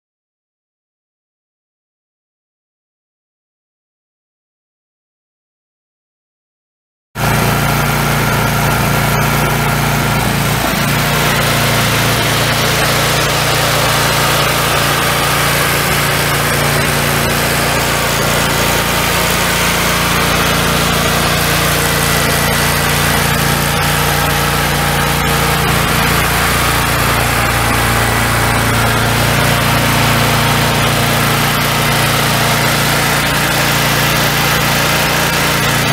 Miller 250NT welder/generator for sale | no-reserve Internet auction September 14, 2017
Miller 250NT welder/generator
518 hours on meter
Onan two cylinder gas engine
20.5 HP
10,000 watts
No leads
Serial LE033634